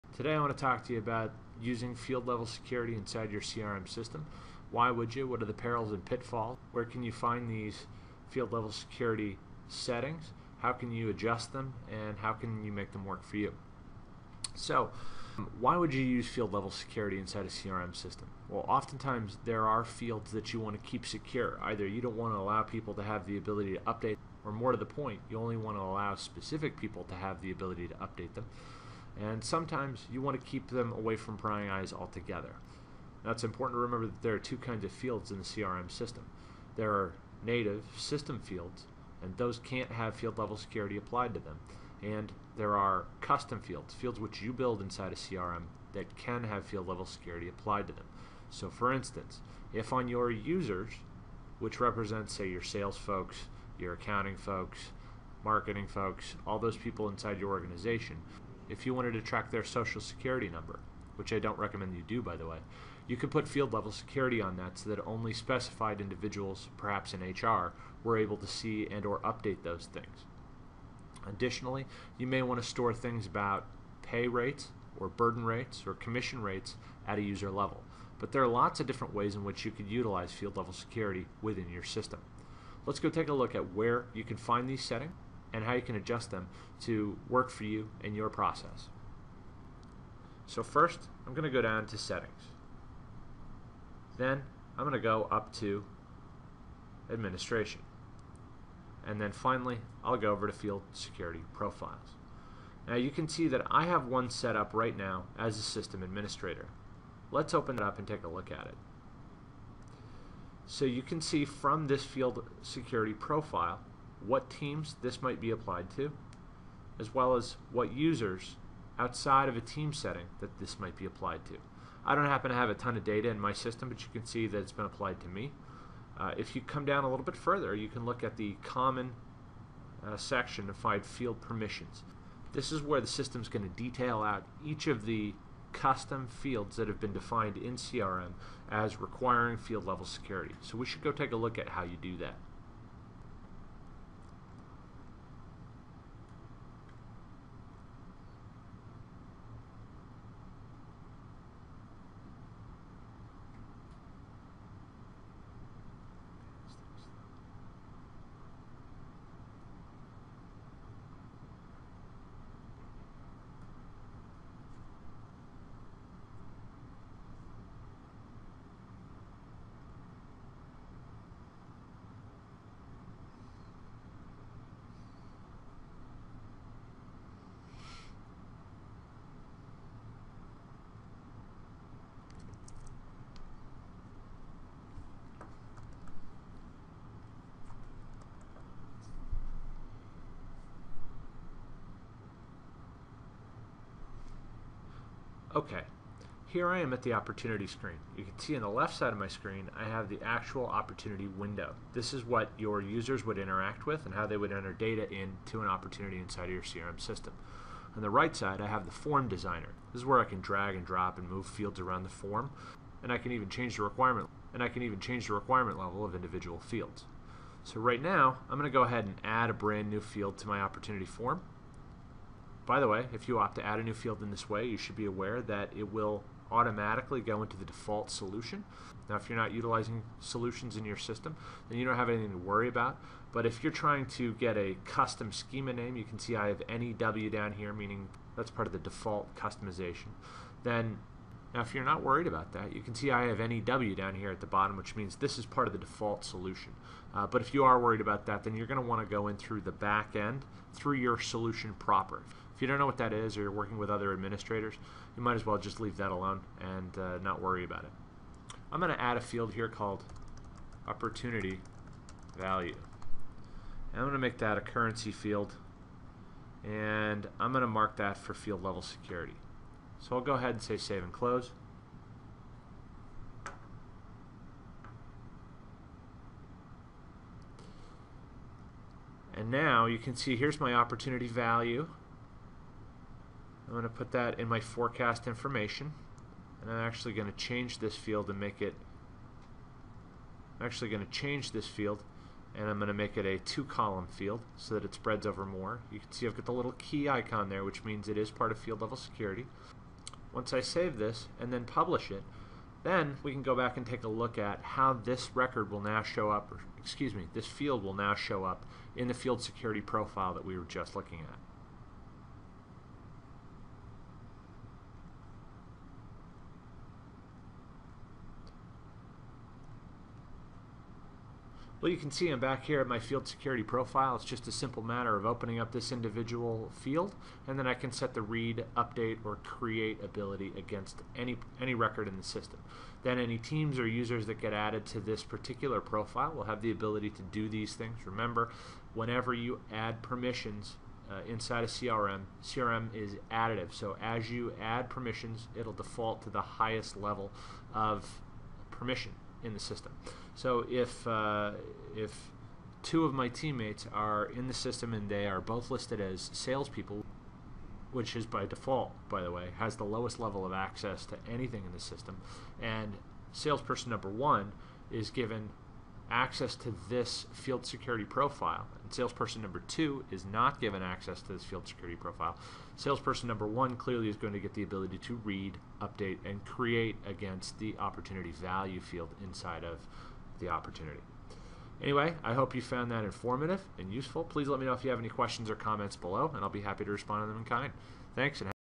How to Use Field Level Security in Dynamics CRM
- Learn why and how to use field level security in Microsoft Dynamics CRM. Avoid some common pitfalls when establishing field level security so that your security settings work with your business processes.